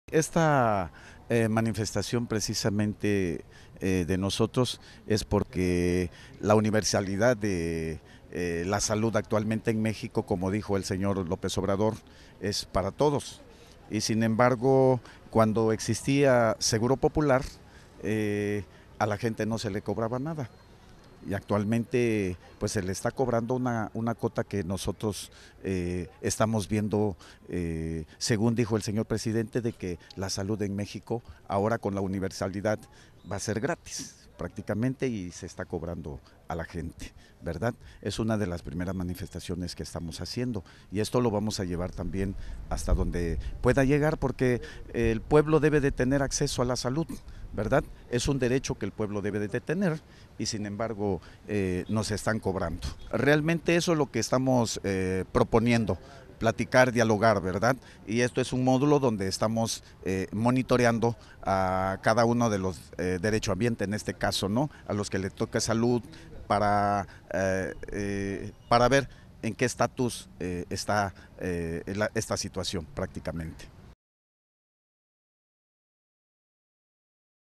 BITE HOSPITAL PROTESTA
Los integrantes de la Red de Salud y Vida en Comunidad de la región del istmo se manifiestan en el acceso principal del Hospital General Dr. Macedonio Benítez Fuentes en Juchitán, para exigir  que no se cobre  por los servicios médicos.
Señalaron que las autoridades de Salud del Estado deben aplicar la política pública que implementa el Gobierno Federal con el Instituto de Salud para el Bienestar, para que los servicios sean gratuitos como lo ha anunciado el Presidente Federal Andrés Manuel López Obrador.
Desde la semana pasada surgió la inconformidad de los usuarios de este Hospital General debido a que se comenzaron a haber cobros por todos los servicios que se ofrecen.
En el acceso los inconformes colocaron una mesa donde estarán recibiendo las quejas para posteriormente acudir con los directivos del hospital, para buscar que no se les cobren los servicios.
Este grupo de personas pertenecen a la Coalición Obrera Campesina Estudiantil del Istmo-Movimiento de Liberación (COCEI-ML).